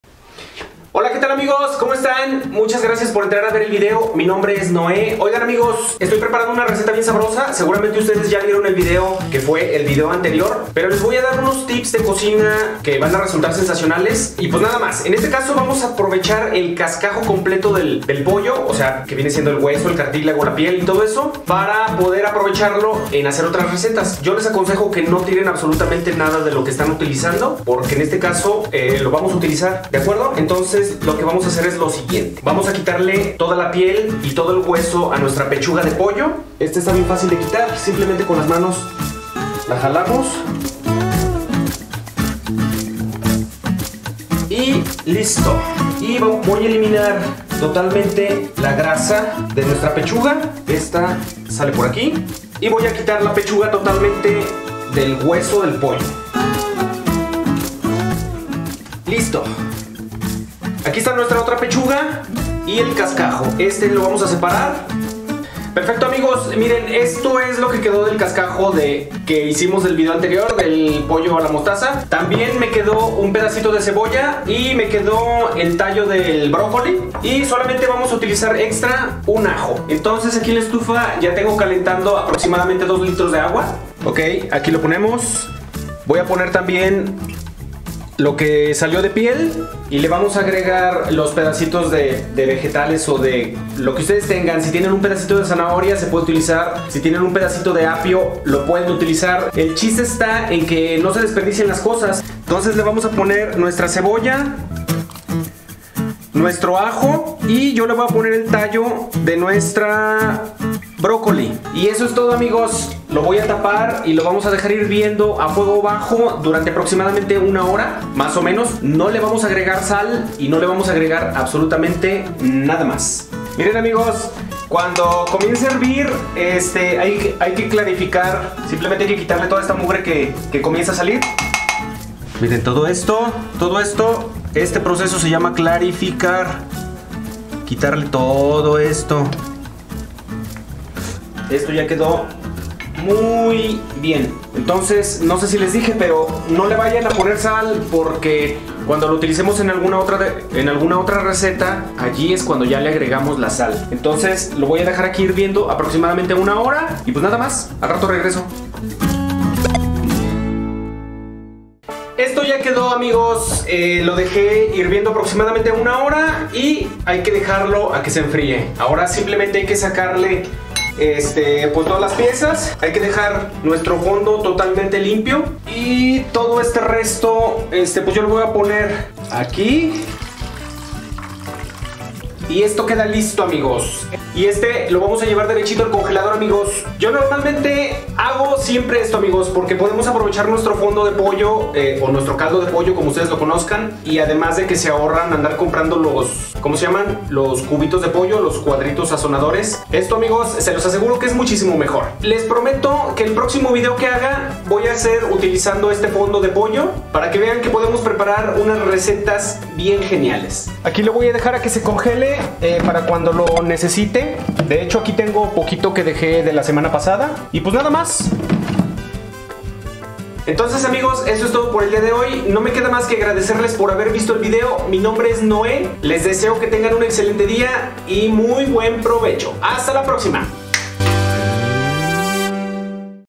Fondo De Pollo Consome De Pollo Caldo de pollo Facil y Rapido Tips De Cocina
No tires los huesos de pollo, en este video te enseño como aprovecharlos al máximo y poder preparar recetas geniales y deliciosas :)

Fondo De Pollo Consome De Pollo Caldo de pollo Tips De Cocina